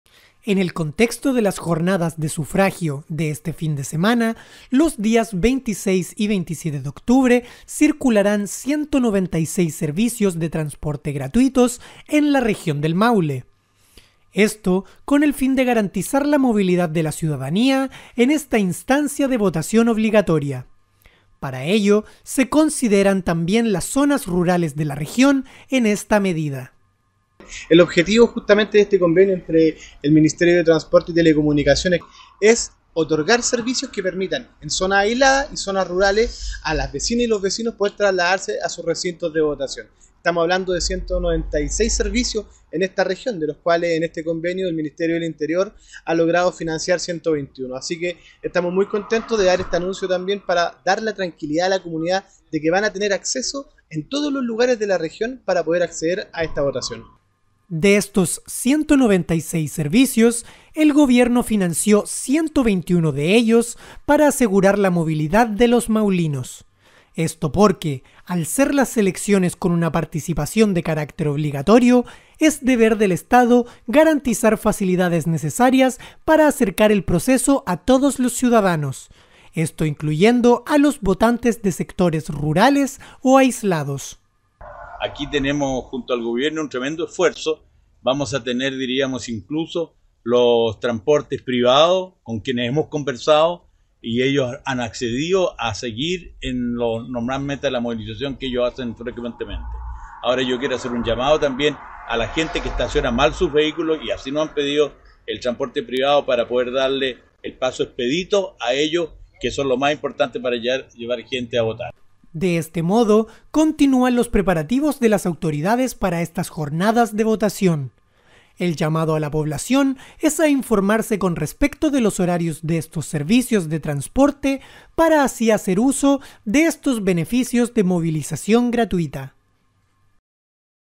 GOBIERNO OFRECERÁ 196 SERVICIOS DE TRANSPORTE GRATUITOS
Con el fin de asegurar la participación ciudadana en este proceso eleccionario, el gobierno dispondrá de 196 servicios de transporte gratuitos en la región. El funcionamiento de estos buses y trenes subsidiados por el Estado, serán monitoreados durante ambas jornadas e incluirán a las comunas rurales de la región.